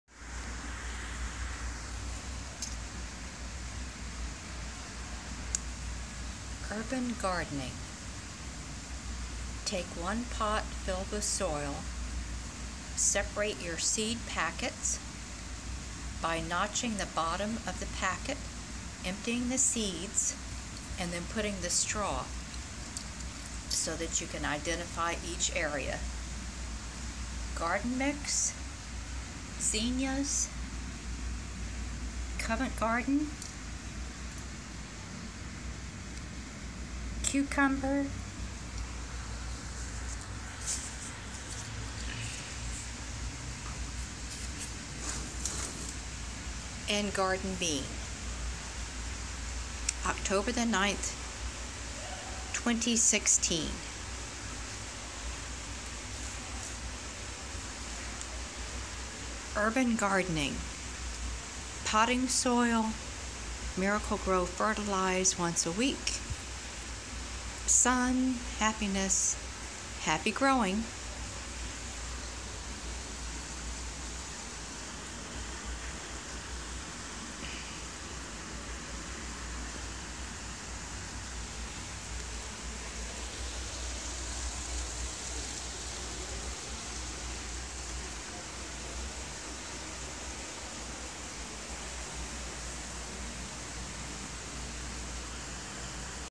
Condo patio container gardening
Seeds planted on a condo patio for container gardening